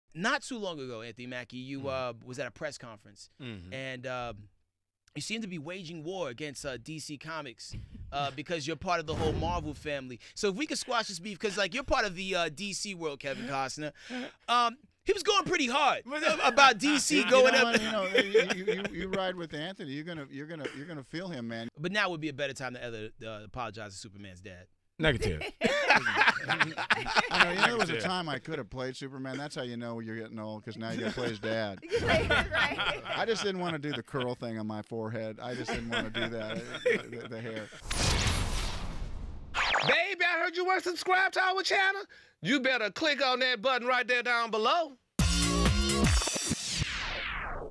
Anthony Mackie and Kevin Costner On DC Vs. Marvel
In this Dish exclusive: Anthony Mackie played Marvel hero Falcon, Kevin Costner played DC character Jonathan Kent (Superman's Earth dad). Now that he's in 'Black or White' with Kevin, will Anthony take back any of the shade he threw at DC?